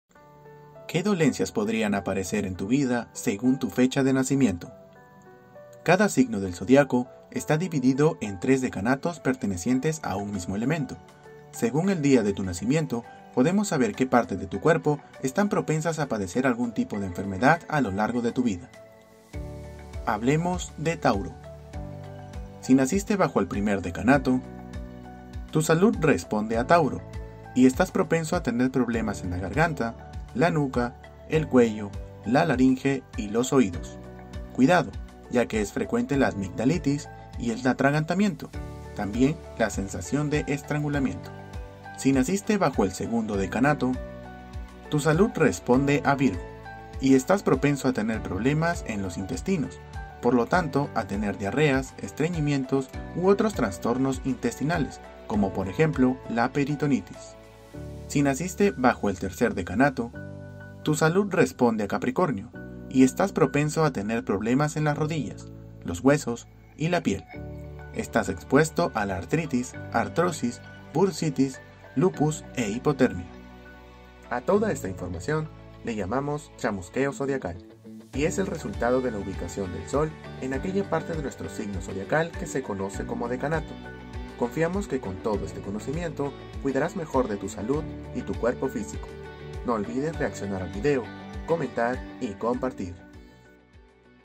No permitas que esos malestares de garganta no te dejen continuar con tu rutina. | Rubén Jungbluth
No permitas que esos malestares de garganta no te dejen continuar con tu rutina. Conoce las partes de tu cuerpo que están más propensas a enfermarse a través de tu Chamusquéo Zodiacal.